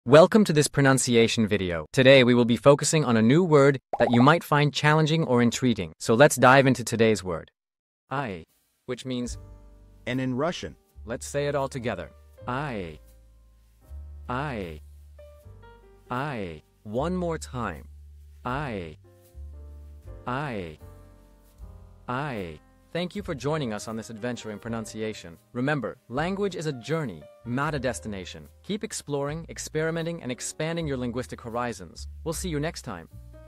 How to pronounce I | I (AND in Russian)
Master the Pronunciation of 'I | I' - which means : AND in Russian 📝 with @PronunciationRussian 🎓🗣️ - [Your Guide to Russian]

Learn to pronounce 'I' perfectly in Russian with our easy and engaging tutorial by @PronunciationRussian! 📖📽️ In this comprehensive video, we provide you with native speaker examples 🗣️👂 and phonetic breakdowns 🎵 to ensure you get the pronunciation just right.